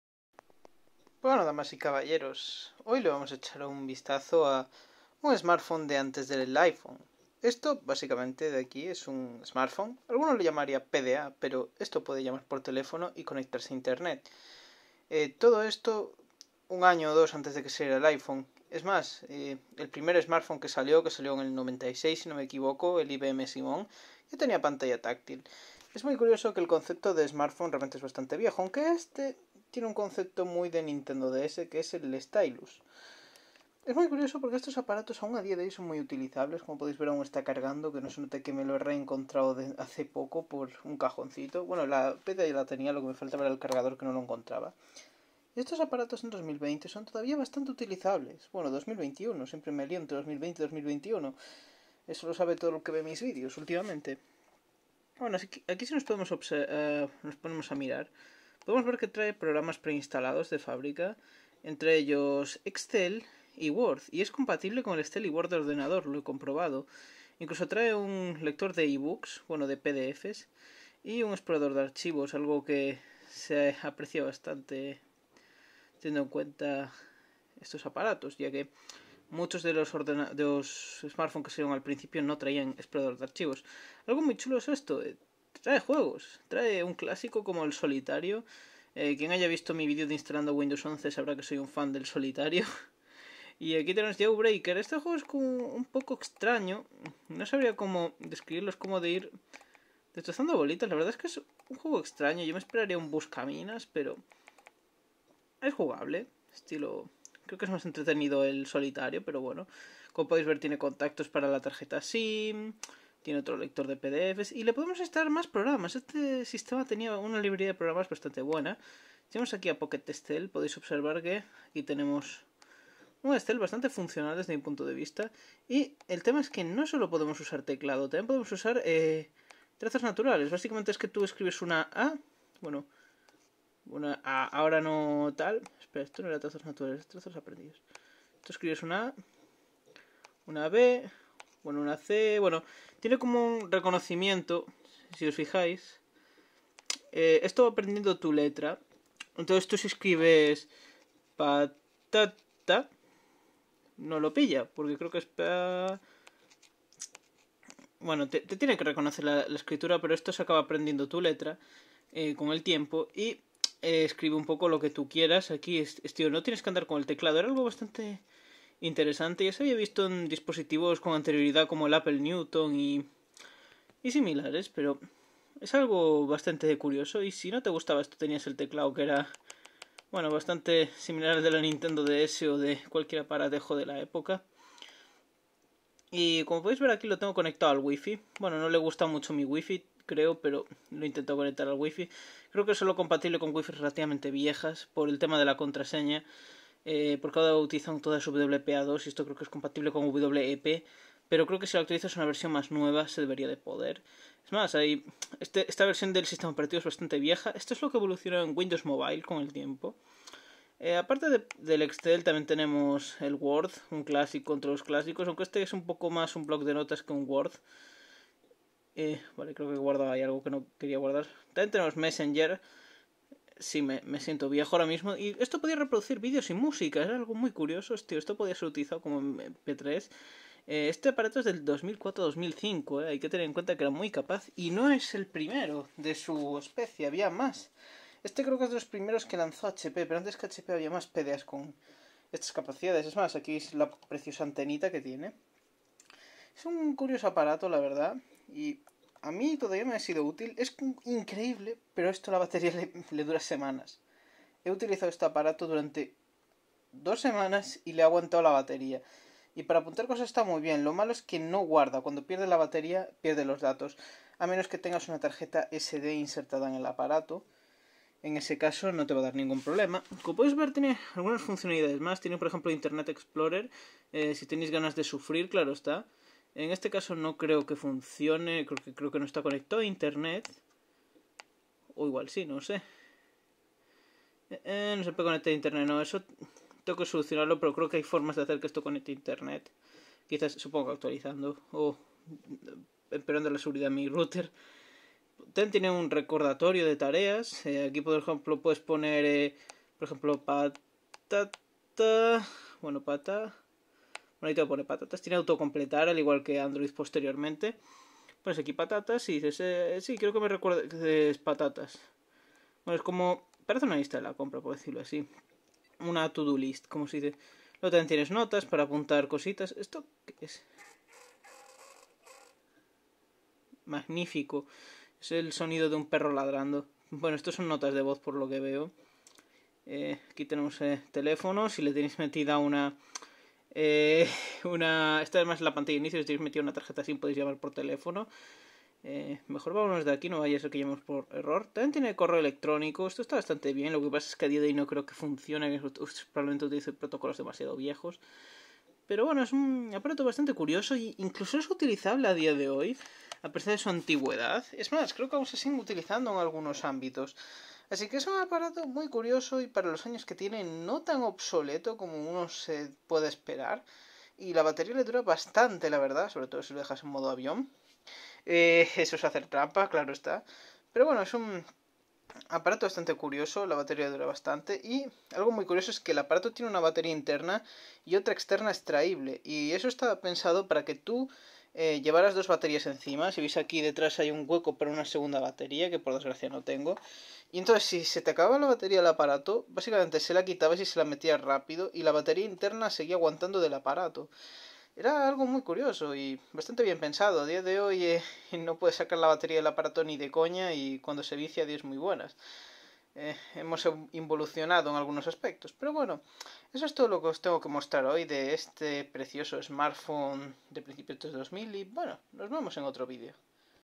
Los SmartPhone antes del iPhone
Hoy le echaremos un vistazo a uno de los smartphone previos al iPhone, cuando Microsoft dominaba este mercado con el sistema operativo Windows Mobile, años antes de IOS y Android.